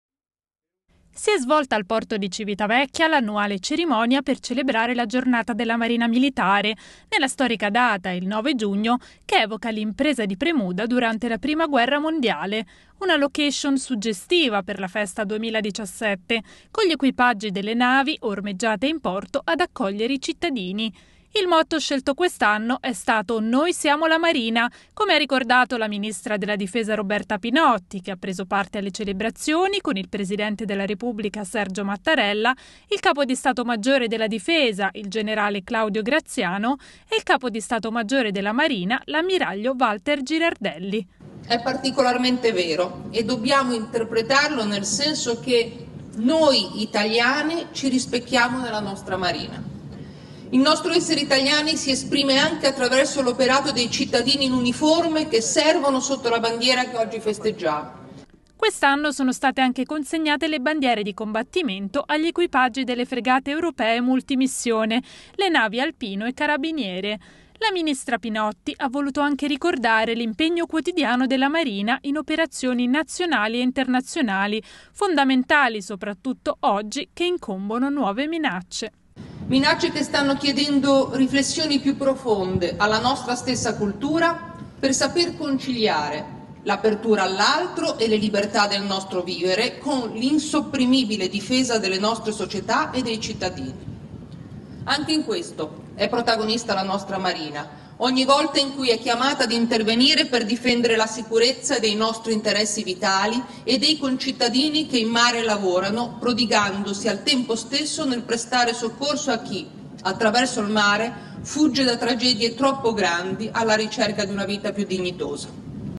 "Noi siamo la Marina", a Civitavecchia la cerimonia del 9 giugno
Roma, (askanews) - Si è svolta al porto di Civitavecchia l'annuale cerimonia per celebrare la Giornata della Marina Militare, nella storica data, il 9 giugno, che evoca l'impresa di Premuda durante la Prima guerra Mondiale. Una location suggestiva per la festa 2017, con gli equipaggi delle navi ormeggiate in porto ad accogliere i cittadini. Il motto scelto quest'anno è stato: "Noi siamo la Marina", come ha ricordato la ministra della Difesa Roberta Pinotti che ha preso parte alle celebrazioni con il presidente della Repubblica Sergio Mattarella, il Capo di Stato Maggiore della Difesa (**parla a 46 circa), il Generale Claudio Graziano, e il Capo di Stato Maggiore della Marina (**parla a 44 circa), l'Ammiraglio Valter Girardelli.
Pinotti: "E' particolarmente vero, e dobbiamo interpretarlo nel senso che noi italiani ci rispecchiamo nella nostra Marina e il nostro essere italiani si esprime anche attraverso l'operato dei cittadini in uniforme che servono sotto la bandiera che oggi festeggiamo".
Quest'anno sono state anche consegnate le Bandiere di Combattimento agli equipaggi delle Fregate Europee Multi Missione (FREMM), le navi Alpino e Carabiniere.
La ministra Pinotti ha voluto anche ricordare l'impegno quotidiano della Marina in operazioni nazionali e internazionali fondamentali soprattutto oggi che incombono nuove minacce.
"Minacce che chiedono riflessioni più profonde per saper conciliare l'apertura all'altro e le libertà del nostro vivere con l'insopprimibile difesa delle nostre società e dei cittadini. Anche in questo è protagonista la Marina. Quando è chiamata a intervenire per difendere la sicurezza dei nostri interessi vitali e dei concittadini che in mare lavorano prodigandosi nel prestare soccorso a chi, attraverso il mare, fugge da tragedie troppo grandi, alla ricerca di una vita più dignitosa".